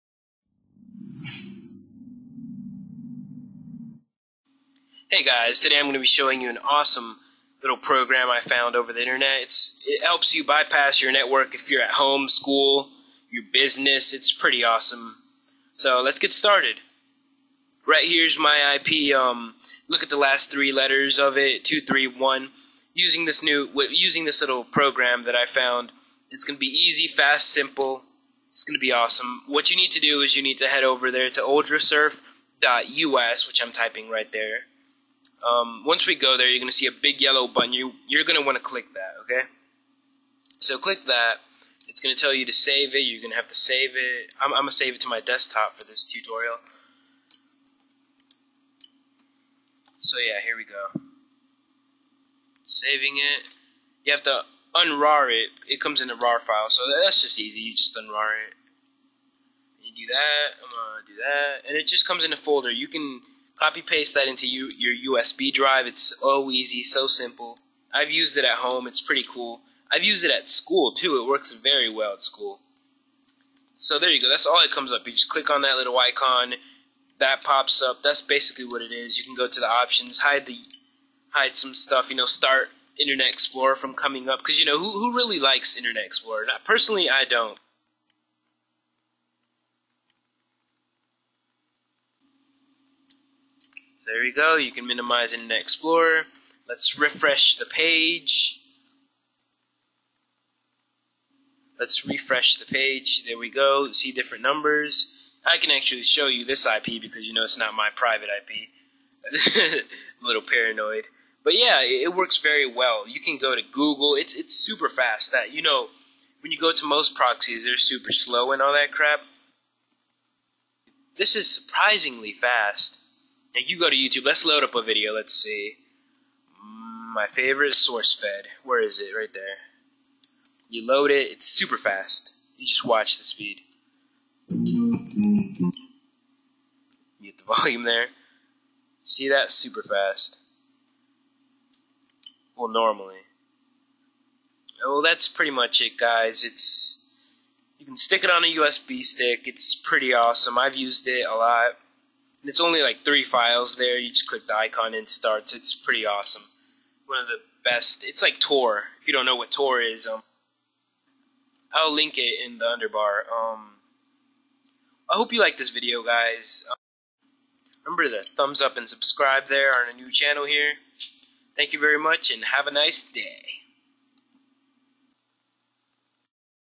How to bypass filters at school, home, and work (EASY+FAST)
Hello guys!
Many times in schools colleges, & offices surfing some sites are banned.
To overmcome this you can unblock them by using proxies or services like TOR.
But there is a wayyy easier way . its this little nifty program called ultrasurf .
Ultra surf is a free software that enables users to visit websites safely and freely through  a secure , encrypted tunnel.

Now i personally have used this software for some time now and can gurantee its abilites. Its suprisingly very fast , unlike other proxies or The Onion Router.
now all we have to do is go to ultrasurf.us
and click on this big yellow button, it will proceed to download the .rar file,
next all you have to do is unrar the file and double click the system and boom your done.
the great thingy about this software is that its fast simple and easy to use.
No installation required.